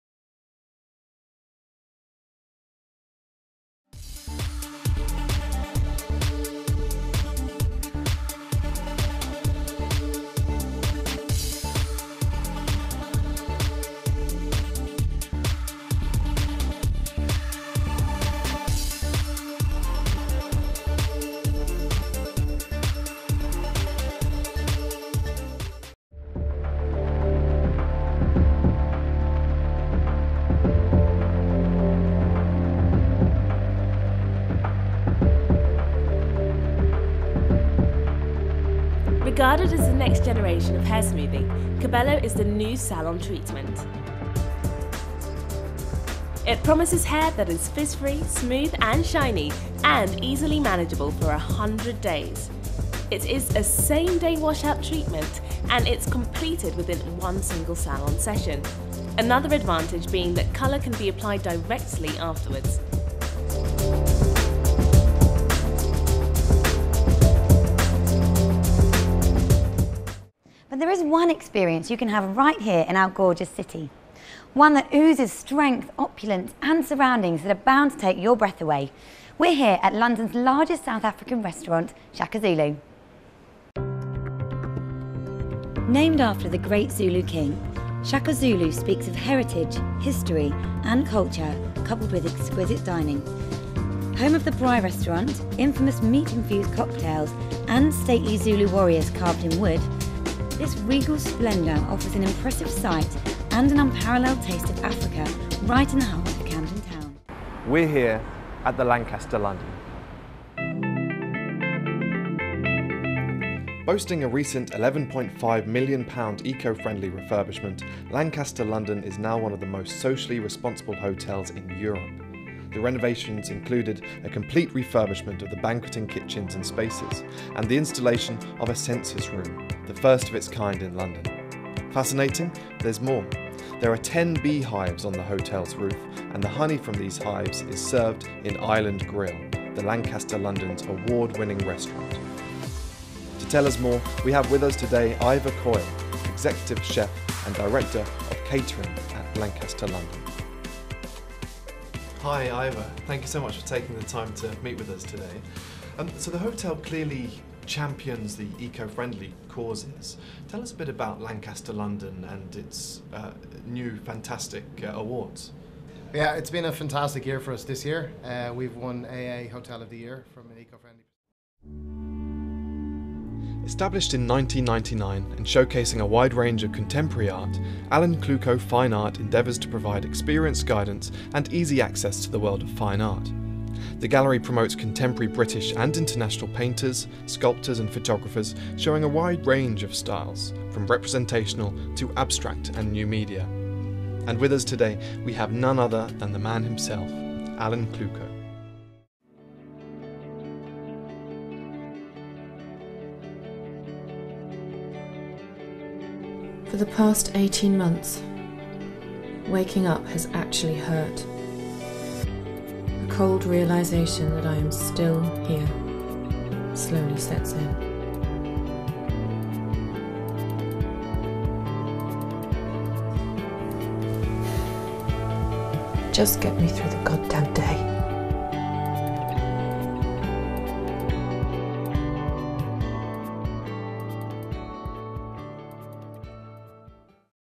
Matmediaonline Production Showreel
Matmediaonline production showreel showcasing their first lifestyle documentary On The Rock. Features top London establishments including Boyd's Brasserie, The Cadogan, Lancaster London and Shaka Zulu. As well as top London hair salon Stonehills.